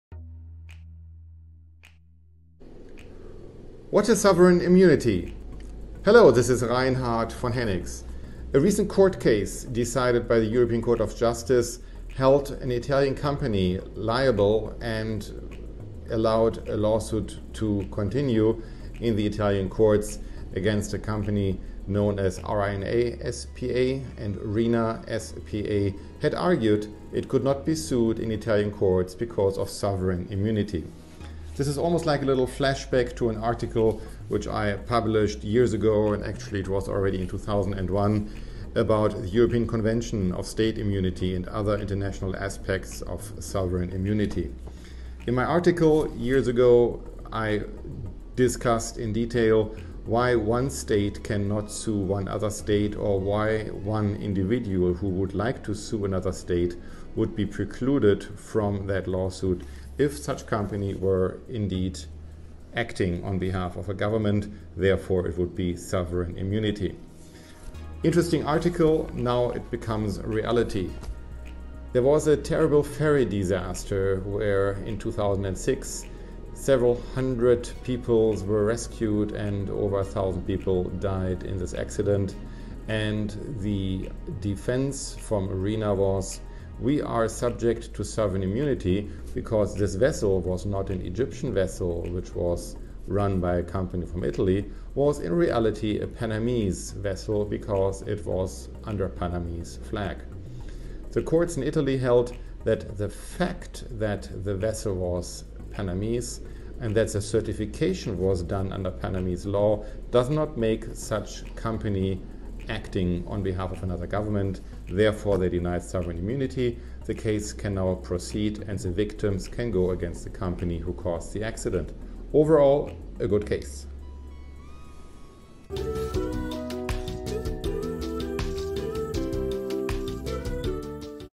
What is sovereign immunity?
Sovereign immunity
Definition: "The sovereign immunity refers to the fact that the government cannot be sued without its consent."
Top EU Court Says Relatives of Ferry Victims Can Sue in Italy.
The family members of more than 1,000 people killed in a 2006 Egyptian ferry disaster can sue for damages in Italy, where the vessel was certified as safe, Europe’s highest court ruled Thursday.
The Al Salam Boccaccio 98 ferry, carrying some 1,400 people between Egypt and Saudi Arabia when it sank, was owned by an Egyptian company and flying Panama’s flag, but the European Court of Justice found that the Italian company that certified the vessel as safe can only claim immunity if it was acting on behalf of the Panamanian government.